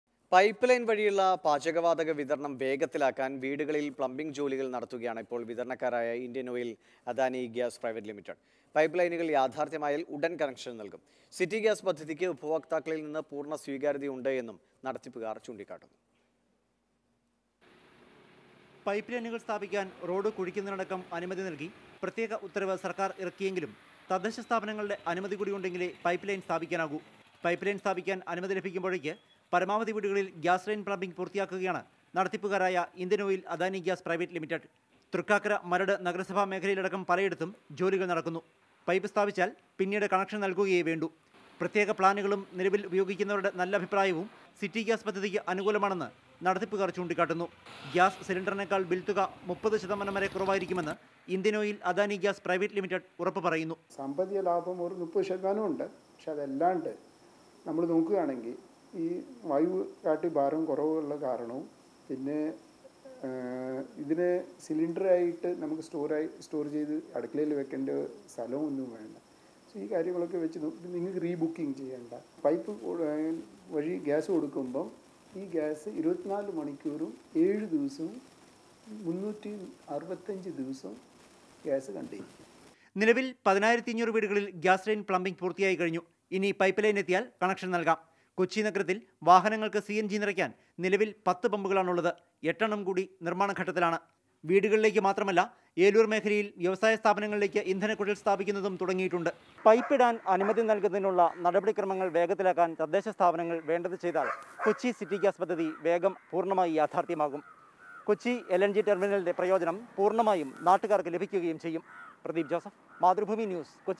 ഗ്യാസ് പൈപ്പ് ലൈൻ പാചകവാതക വിതരണം ഇഴയുന്നു | Gas Pipe Line | Adani Group
പൈപ്പ് ലൈൻ വഴിയുളള പാചകവാതക വിതരണം വേഗത്തിലാക്കാൻ വീടുകളിൽ പ്ലംബിങ് ജോലികൾ  നടത്തുകയാണ് വിതരണക്കാരായ ഇന്ത്യൻ ഓയിൽ  അദാനി ഗ്യാസ് പ്രൈവറ്റ് ലിമിറ്റഡ് .പൈപ്പ് ലൈനുകൾ യാഥാർഥ്യമായാൽ ഉടൻ കണക്ഷൻ നൽകും.

Happy viewing!